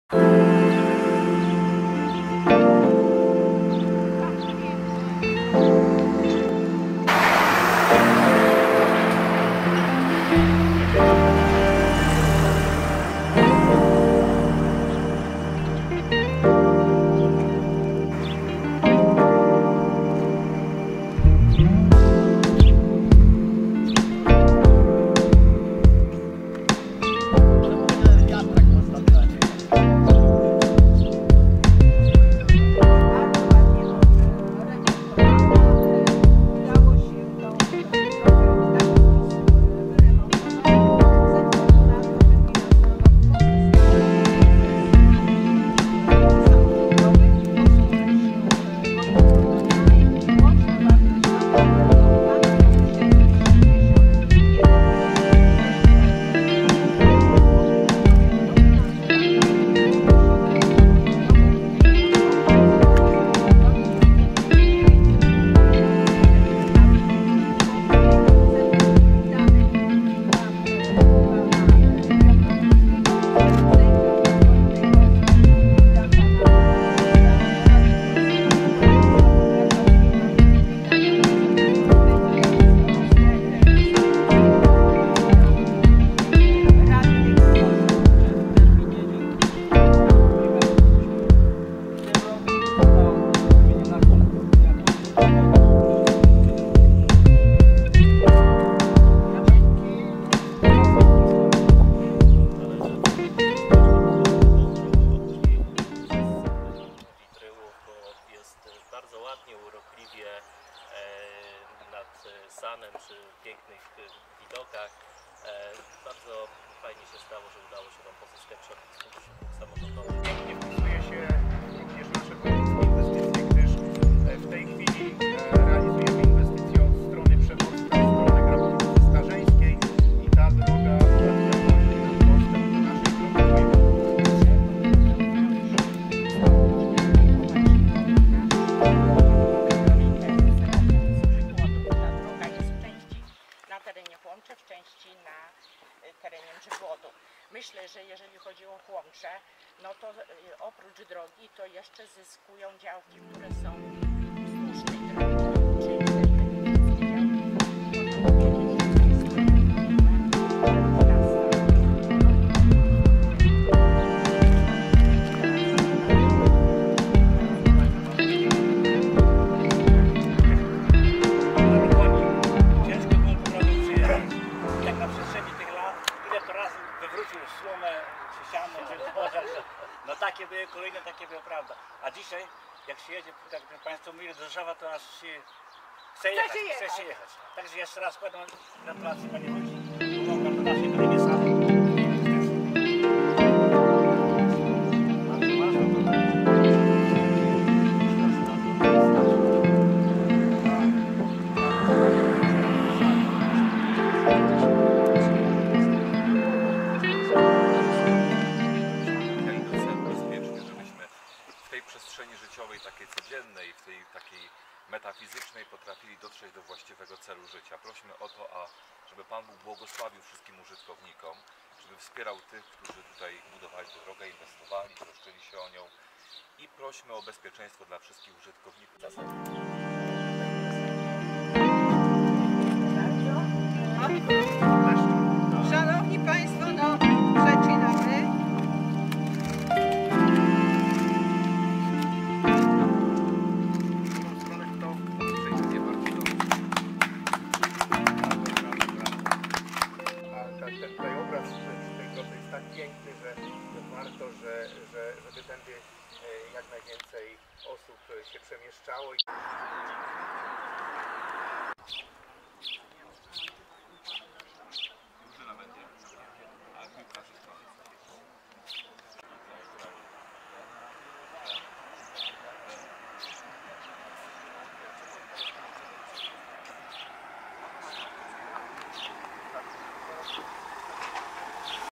Droga Mrzygłód Końskie Otwarcie
W pierwszym filmiku pokazałem przebudowę drogi a teraz zapraszam do obejrzenia urzędowego otwarcia i można śmigać przestrzegając zasad ruchu drogowego.